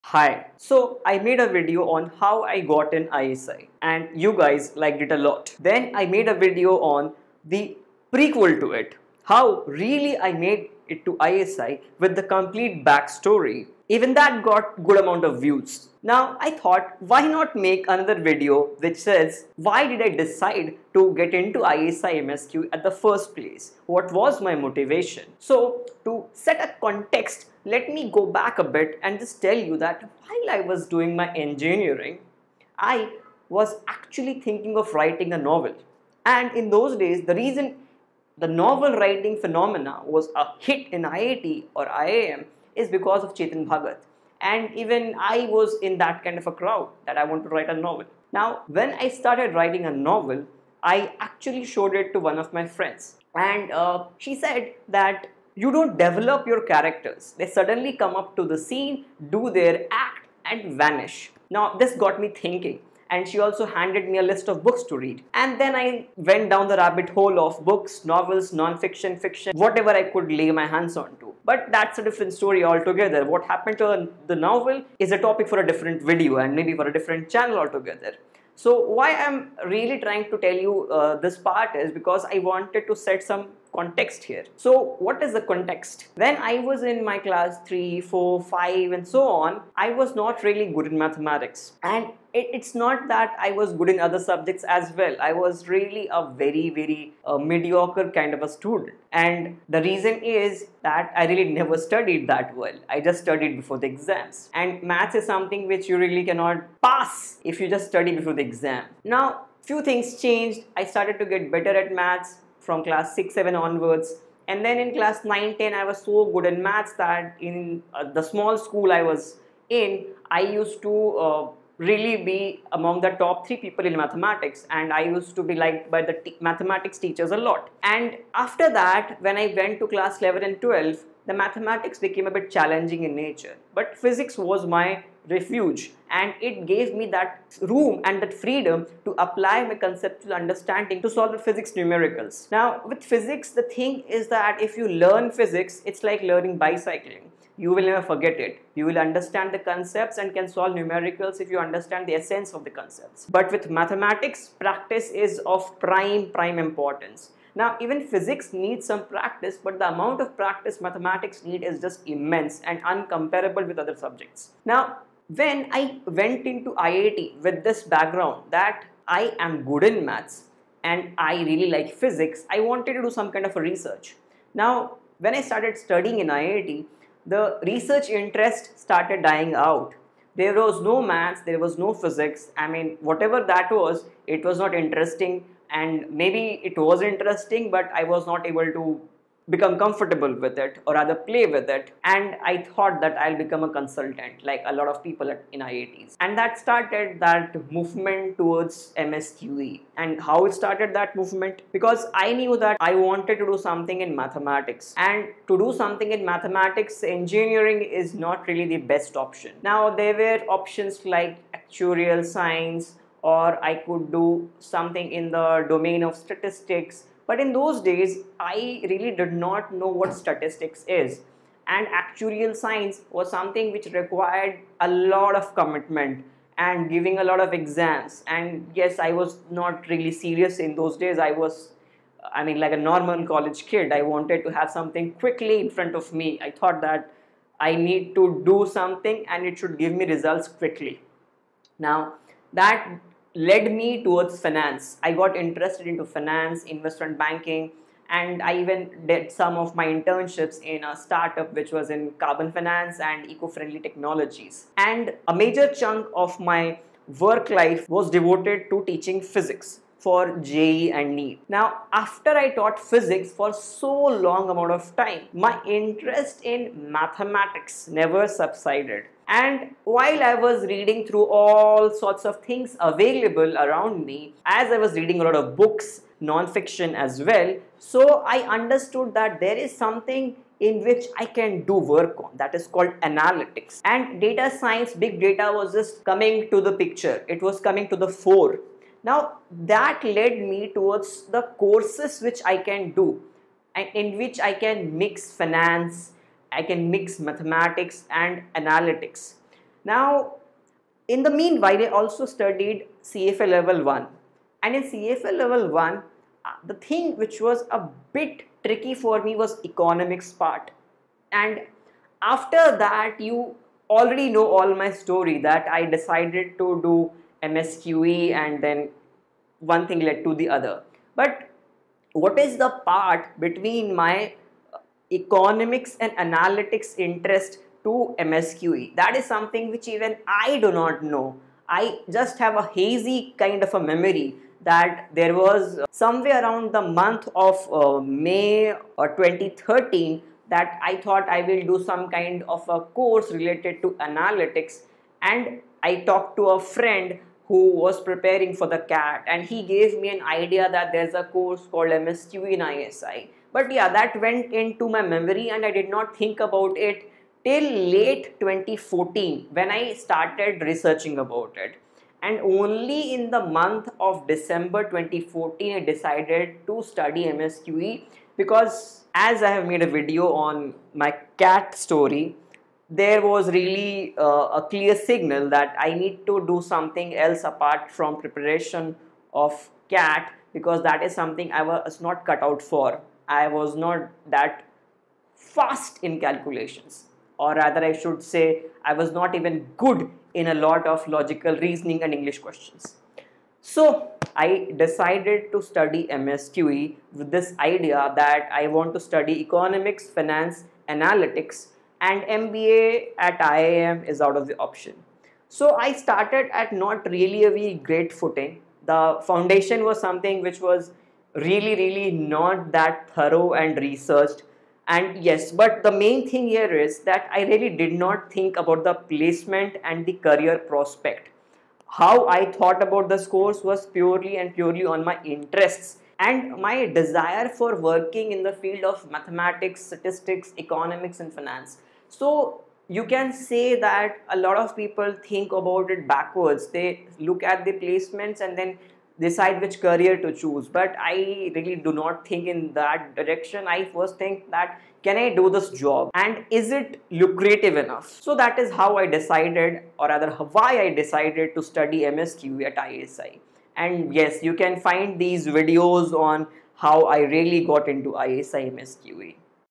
Why I Decided to Study MSQE at ISI Kolkata
Why I chose ISI MSQE for my Masters? [Blog]
Which is better? ISI Delhi Vs ISI Kolkata Vs DSE? But before you answer that you need to decide whether you want to study 'Quantitative Economics' or 'Masters in Economics'. So, Why would one choose a course over the other? I don't answer that question in this video but I give you the context of 'Why I chose ISI MSQE for my Masters'
Tomato (Test of Mathematics at the 10+2 Level )
Statistics (I'll be adding more books in future)
Econometrics (Statistics for ISI / DSE)

Yours Truly,
Ashish Gourav